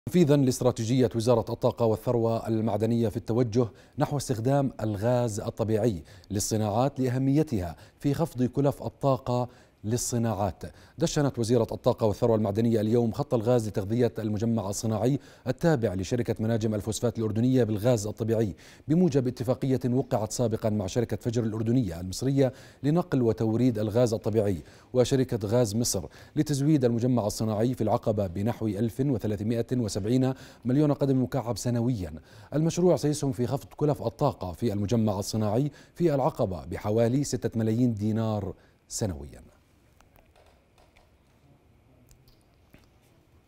تدشين خط الغاز لتغذية المجمع الصناعي في العقبة
دشنت وزيرةُ الطاقة والثروة المعدنية اليوم خط الغاز لتغذيةِ المجمع الصناعي التابع لشركة مناجم الفوسفات الأردنية بالغاز الطبيعي،تنفيذا لاستراتيجيةِ وزارة الطاقة والثروة المعدنية في التوجه نحوَ استخدام الغاز الطبيعي للصناعات لأهميتها في خفضِ كلف الطاقة للصناعات